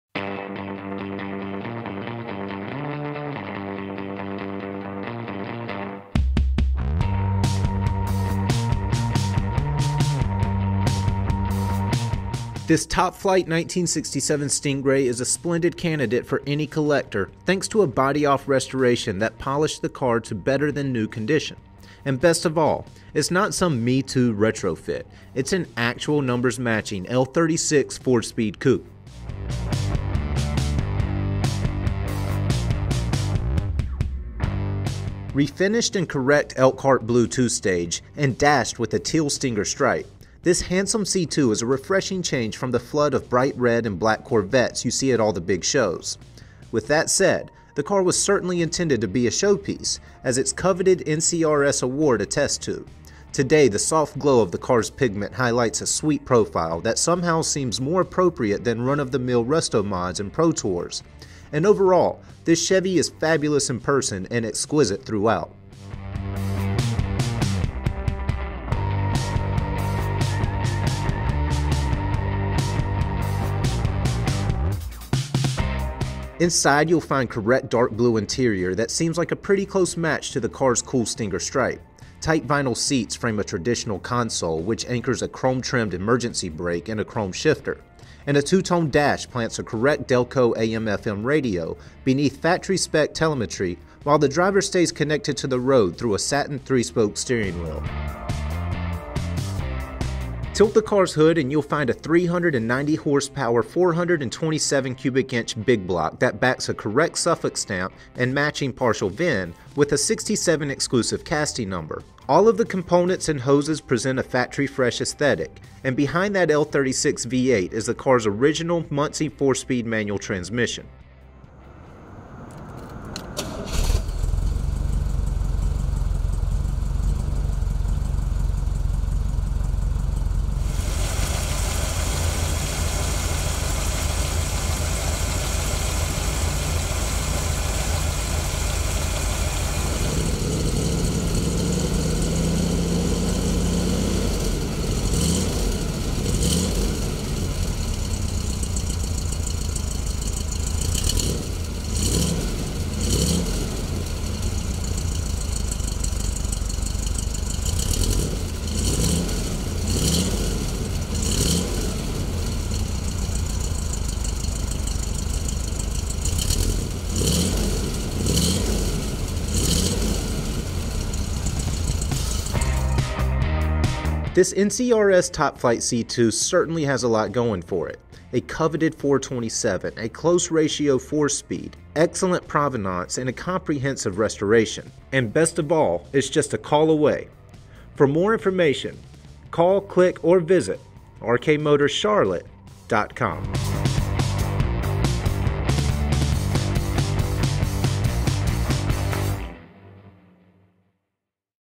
135660 / 1967 Chevrolet Corvette Sting Ray
This Top Flight Sting Ray is a splendid candidate for any collector thanks to a body-off restoration that polished the car to better-than-new condition. And best of all, it's not some me-too retrofit, it's an actual numbers-matching L36/4-speed coupe!

BODYWORK/TRIM

Refinished in correct Elkhart Blue 2-stage, and dashed with a cool Teal Stinger Stripe, this handsome C2 is a refreshing change from the flood of bright red and black Corvettes you see at all the big shows. That said, the car was certainly intended to be a showpiece, as it's coveted NCRS award attests to. And one look at our high resolution photos proves the ground-up rebirth, during which the car was separated from its chassis and fully disassembled, was both thorough and professional. Of course, restorations are generally much more successful when they begin with a solid foundation. And since this Vette was parked in 1980 and essentially preserved until its pre-2010 refurb, it certainly proved an excellent starting point. Today, the soft glow of the car's pigment highlights a sweet profile that somehow seems more appropriate than run-of-the-mill resto-mods and pro-tourers. And overall, this Chevy is fabulous in person and exquisite throughout.

ENGINE

Tilt the car's hood and you'll find a 390 horsepower, 427 cubic inch big block that backs a correct Ð-¨ILÐ-¨ suffix stamp and matching 111564 partial VIN with a '67-exclusive 3904351 casting number. At the top of the Chevy Orange mill, a familiar Holley carburetor rides between a chrome, correctly decaled air cleaner and bright stainless fuel line. Below that carb, a color-keyed intake funnels a perfect air/fuel mixture in to tough, cast iron heads, which are capped by stamped and painted valve covers. When the juice hits the cylinders, reliable, chrome-capped ignition sparks fresh, stainless-sleeved wires while a stock cam and rotating assembly push spent gases through oversprayed exhaust manifolds. And cooling for that fire is provided by a beefy radiator, which hangs in front of pliable GM hoses and reproduction tower clamps.

DRIVETRAIN/SUSPENSION

Behind that awesome big block, a tough Muncie 4-speed sports a correct 3885010 casting number, a Muncie (P) assembled on February (B) 3rd (03), 1967 (7) build stamp and a matching 111564 partial VIN. That original transmission twists a factory-correct rear end that's fitted with a posi-traction differential and spry, 3.55 gears. Turns come courtesy of standard manual steering. Four disc brakes provide solid, confident stops. And all that reliable hardware rolls on conservative Rally Wheels that spin 7.75-15 BF Goodrich Silvertowns around stainless trim rings and polished center caps.

INTERIOR

Inside this Chevy you'll find a correct Dark Blue interior that, aesthetically, seems like a pretty close match to the car's cool Stinger Stripe. Tight vinyl seats frame a traditional console, which anchors a chrome-trimmed e-brake behind a requisite chrome shifter. In front of that console, a 2-tone dash plants a correct Delco AM/FM radio beneath factory-spec telemetry. And the driver stays connected to the road through a traditional, satin-spoke steering wheel.

This NCRS Top Flight C2 certainly has a lot going for it: a coveted 427, a close-ratio 4-speed, excellent provenance and a comprehensive restoration. And best of all, it's just a call or click away! for more information.